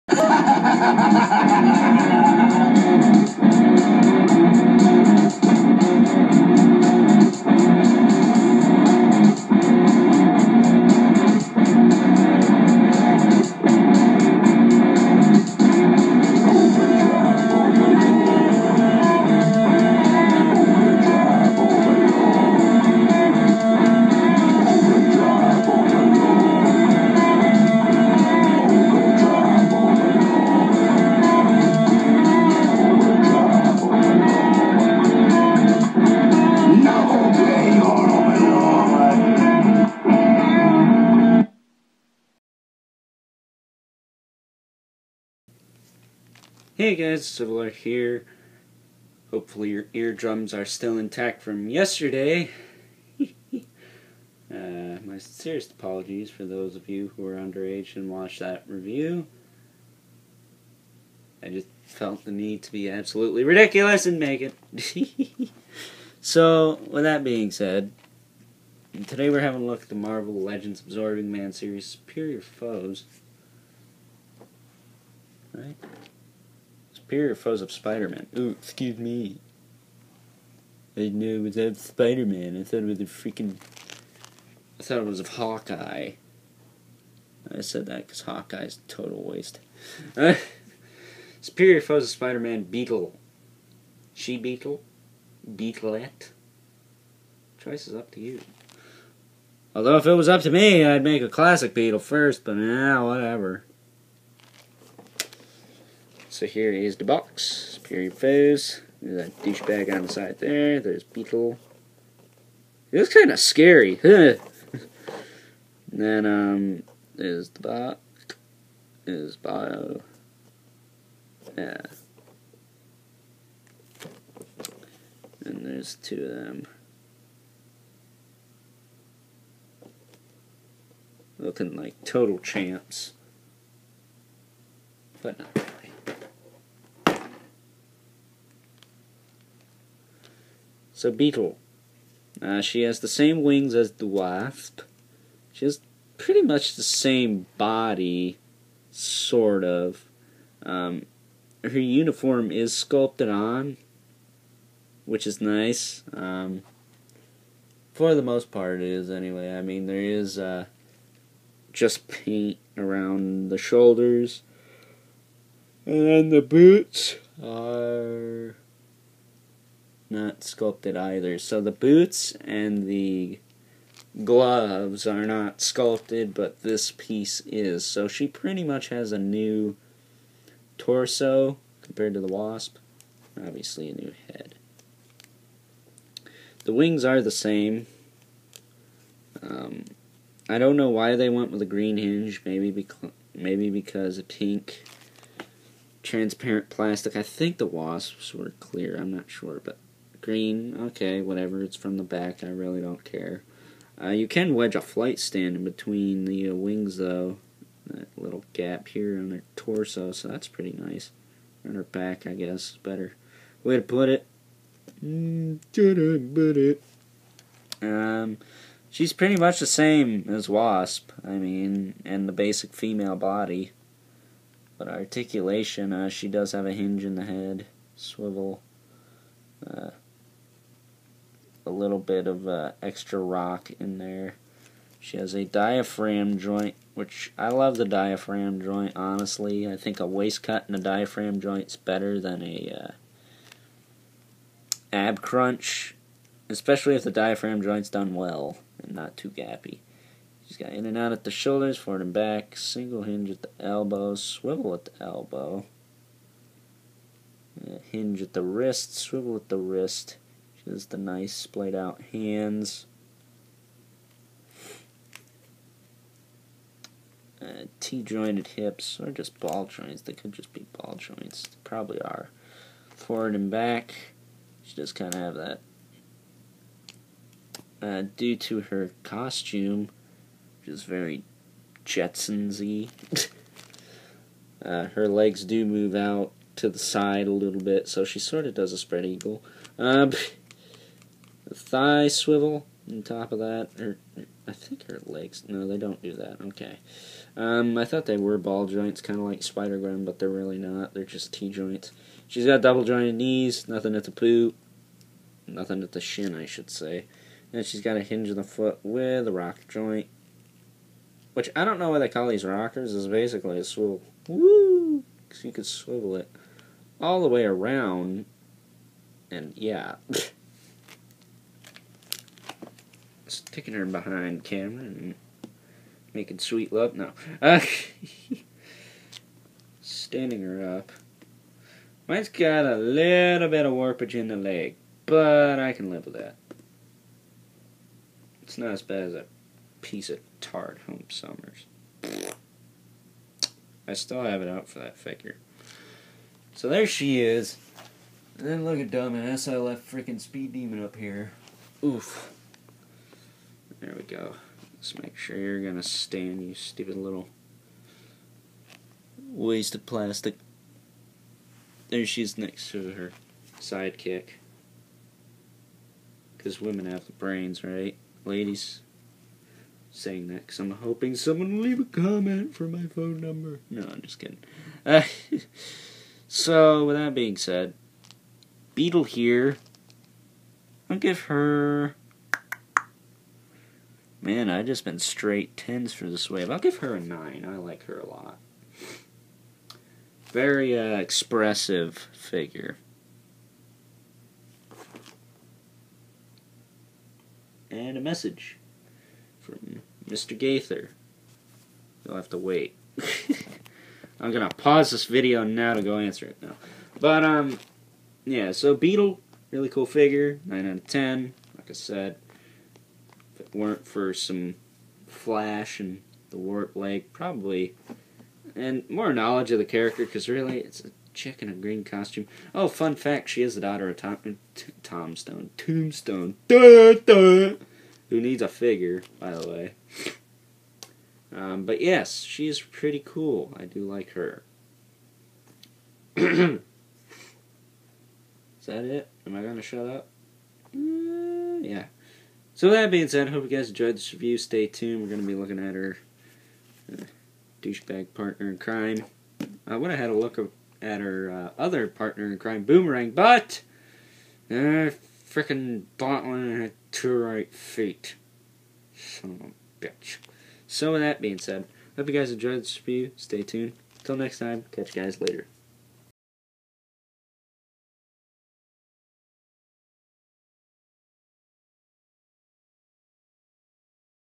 Marvel Legends Absorbing Man Series Superior Foes Of Spider-Man Beetle
A review of the Marvel Legends Absorbing Man Series Superior Foes Of Spider-Man Beetle.